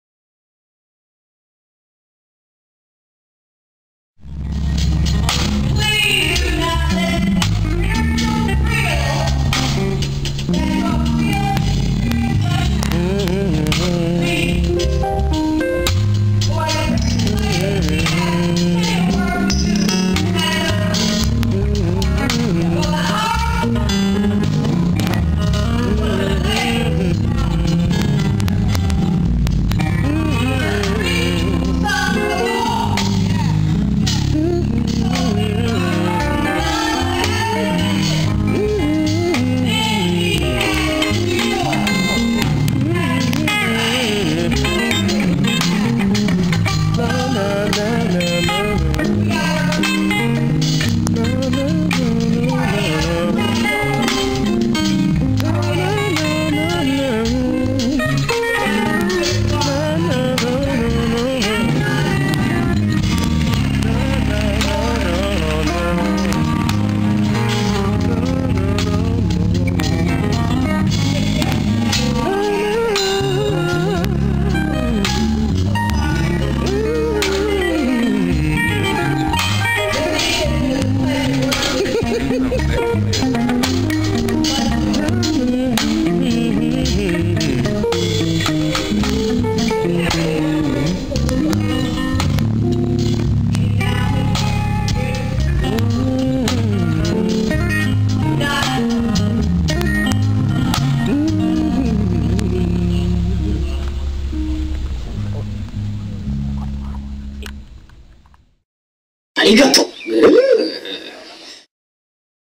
Musician's Corner, Palm Service: Javier Following the Singer
The cameraman wants some too? What is WRONG with y'all boyz, man!? It's like a virus; music is our EPIDEMIC!